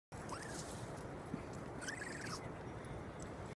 Cute chihuahua sneeze
This is a cute little clip of my chihuahua Coconut sneezing 😊